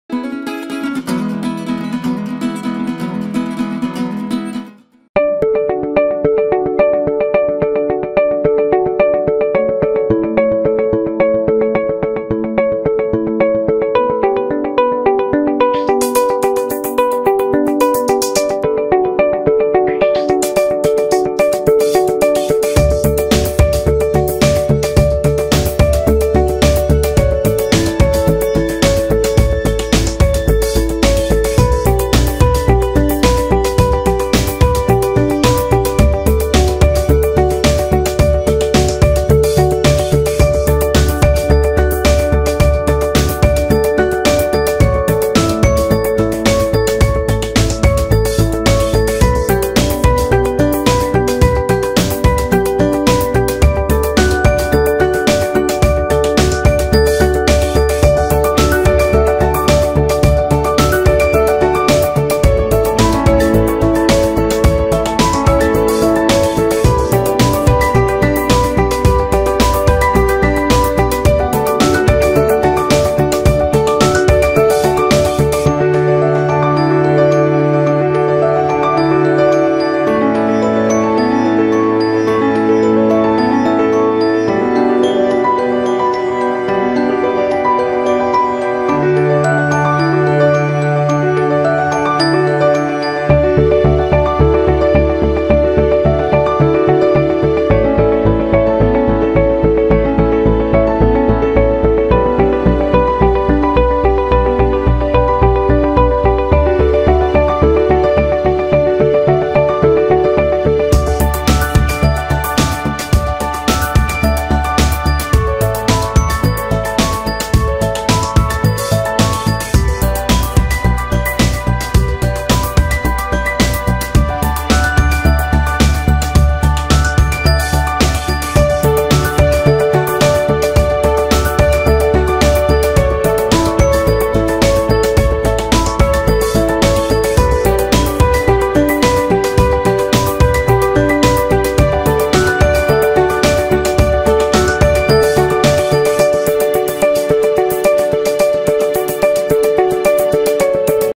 Time lapse of 20 year old leather farm boots being polished
Short time lapse video of polishing 20 year old leather boots